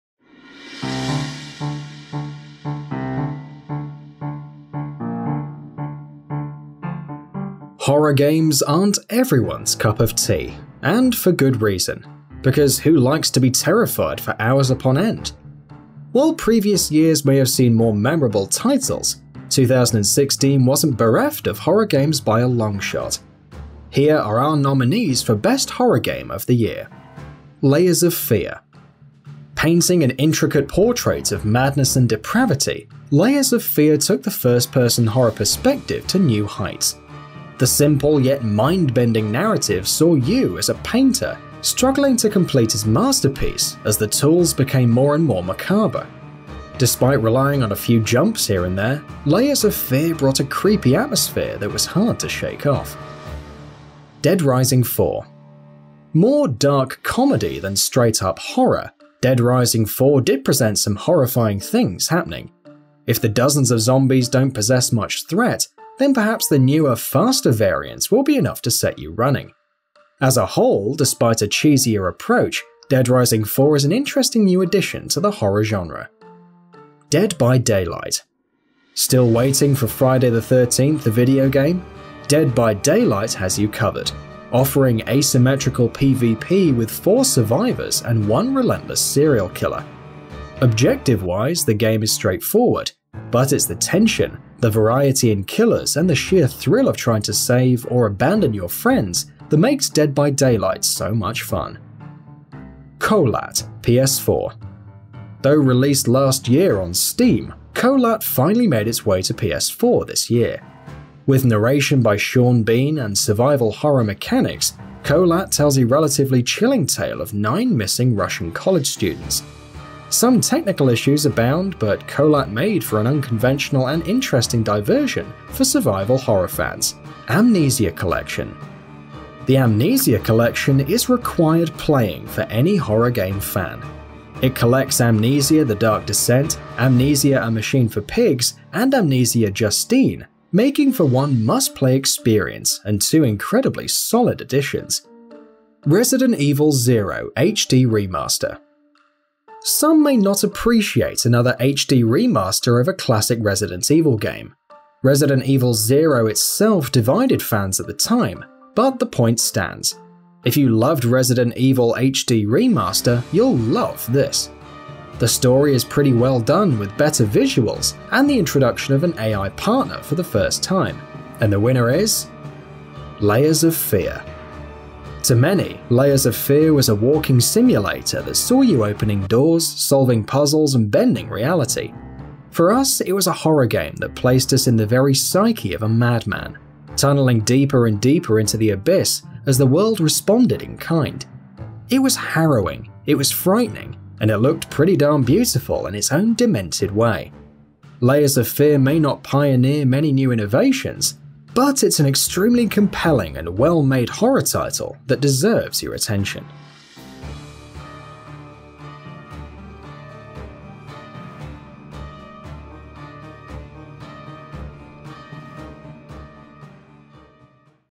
6 Best Horror Games of 2016
Horror games aren't everyone's cup of tea – and for good reason because who likes to be terrified for hours upon end? While previous years may have seen more memorable titles, 2016 wasn't bereft of horror games by a long shot.